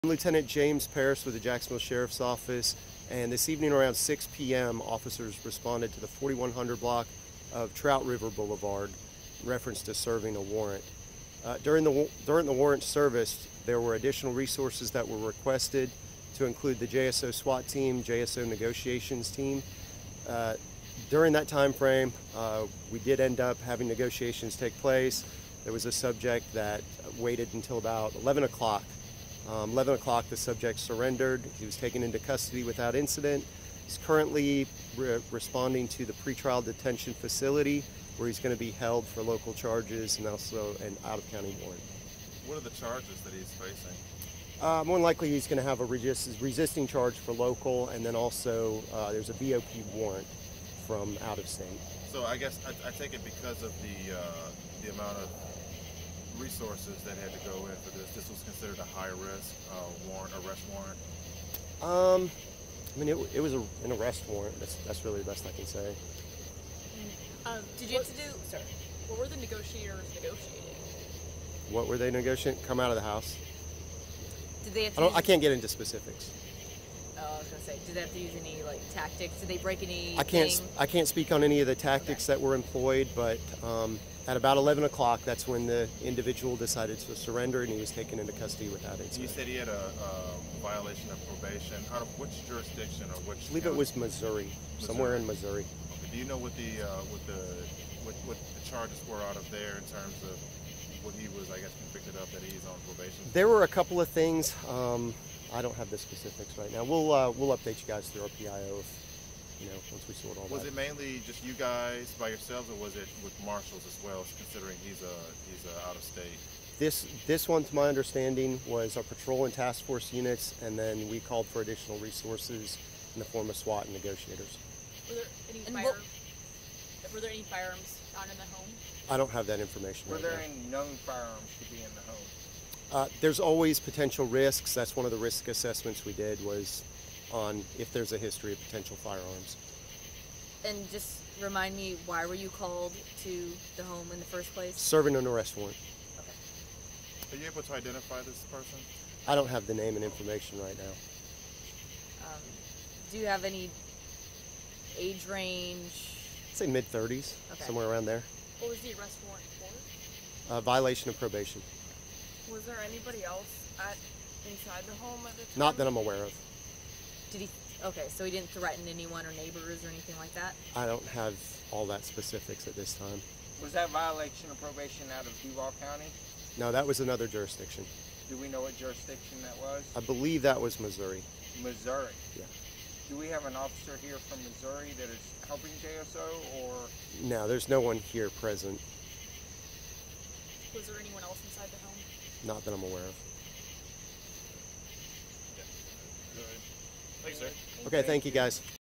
Suspect surrenders peacefully after hours-long standoff in Northwest Jacksonville
Police came to the home in the 4200 block of Trout River Boulevard to serve an arrest warrant for a man wanted for probation violation outside of Florida.